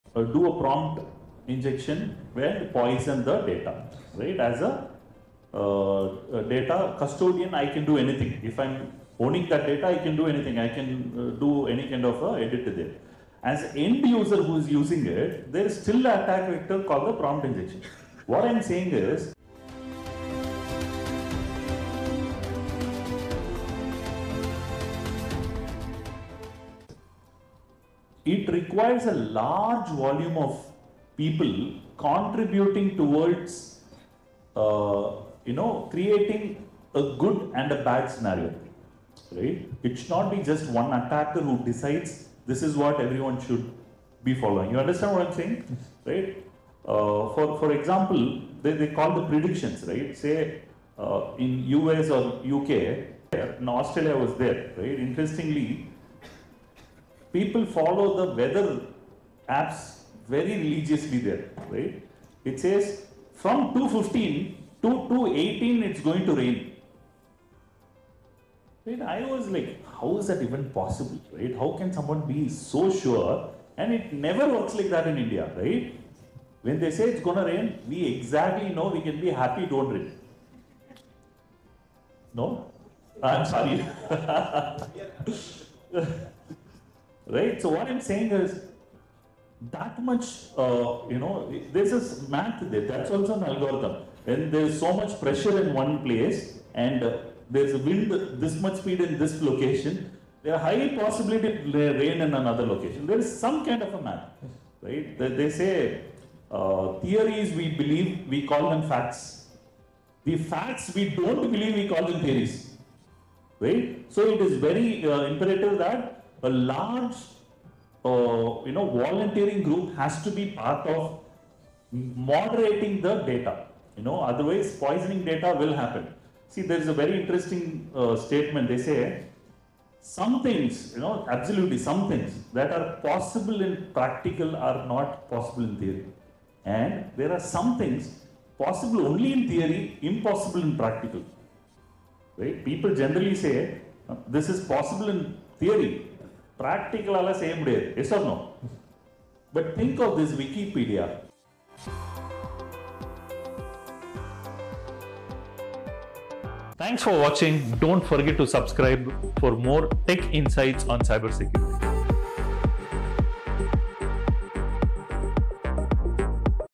Innovating with AI | Amrita Engineering College Guest Event | PART-8(239/365) - Vinod Senthil
Step into the world of Artificial Intelligence with me as I attend an exclusive event at Amrita Engineering College. As a special guest, I explore groundbreaking AI innovations and join thought-provoking discussions on the future of technology. This video captures key moments from presentations, workshops, and interactions with experts and students. Get ready to dive deep into the possibilities of AI.